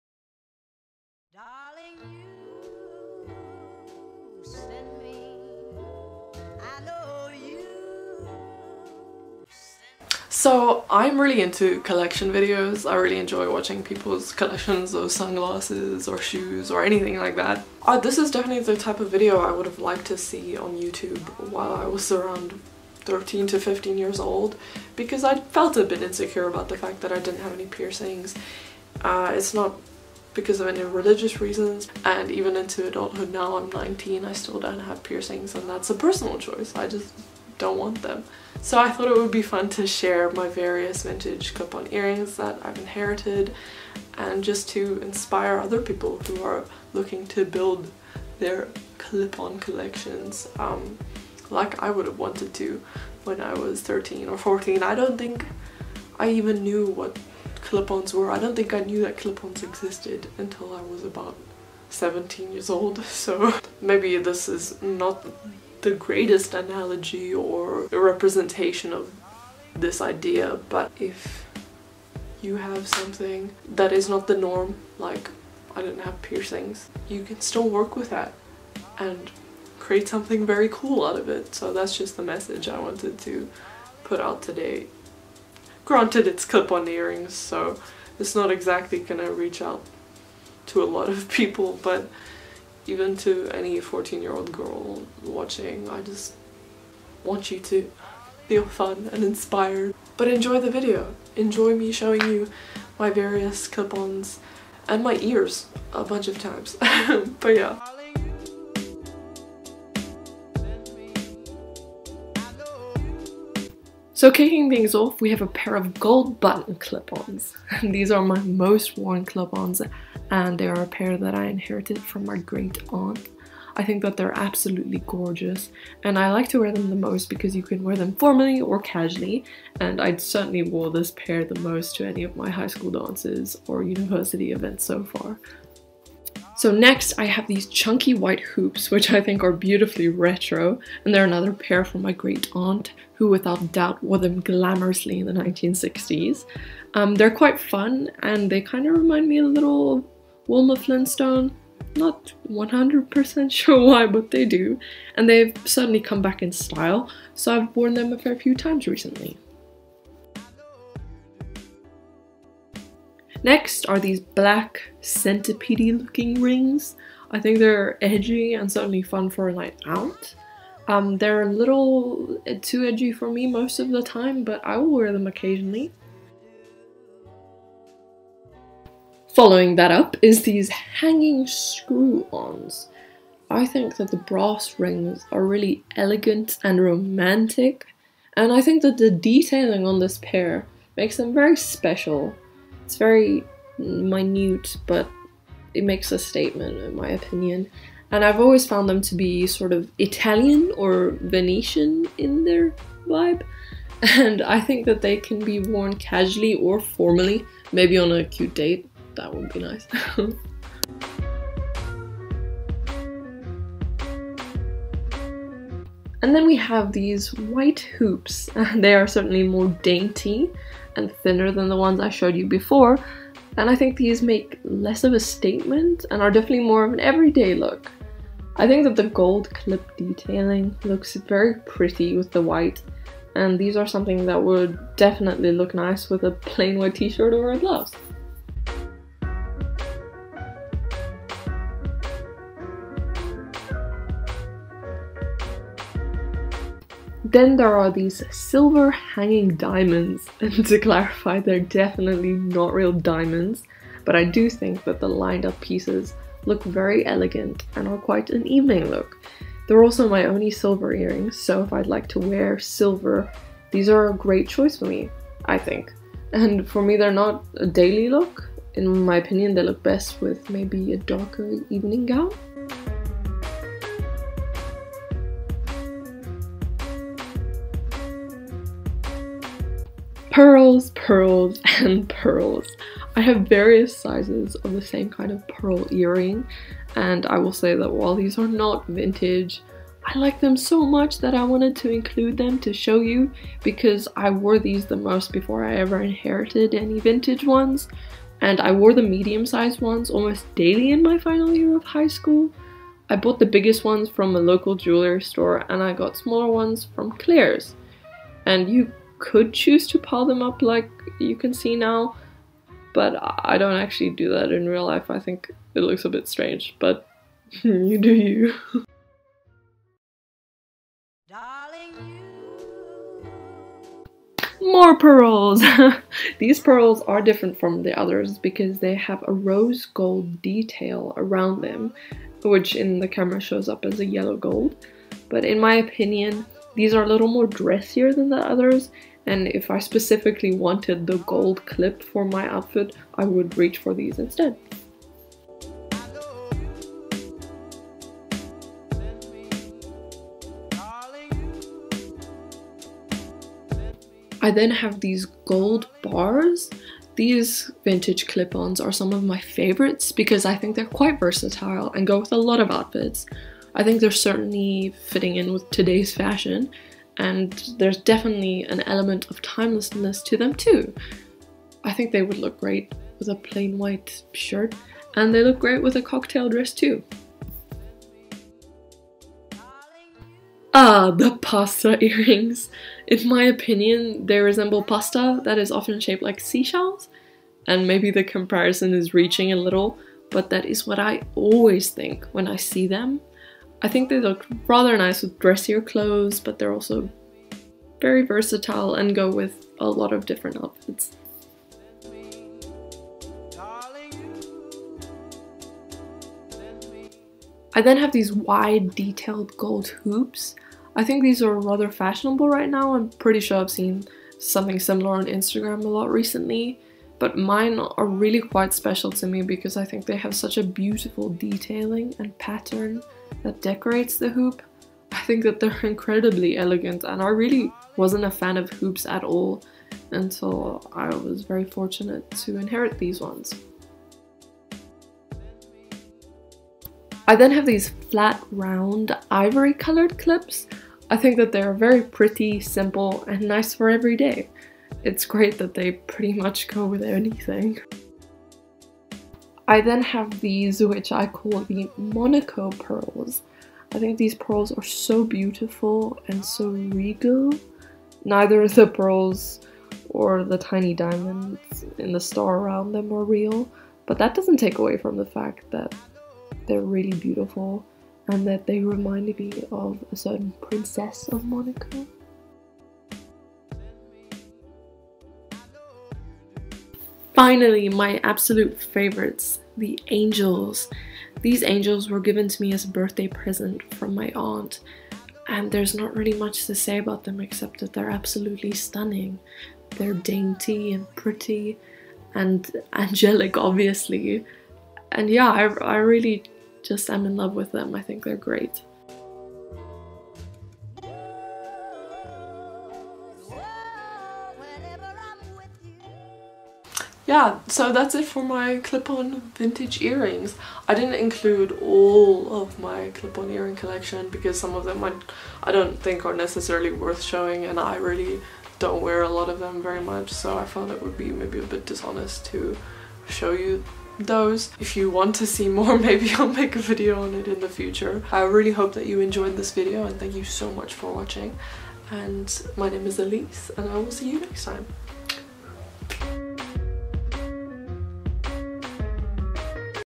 My Vintage [NO Piercings] Earring Collection ✨ Clip-ons
Hey, I don't have piercings. But i'm mighty lucky to have inherited [and bought] several BEAUTIFUL clip-on earrings. They don't require any piercings! I thought I'd share my collection with the world because it's a video I would have very much liked to see pop up for me when I was a younger teen. Enjoy, and let me know which one was your favourite down below! ♥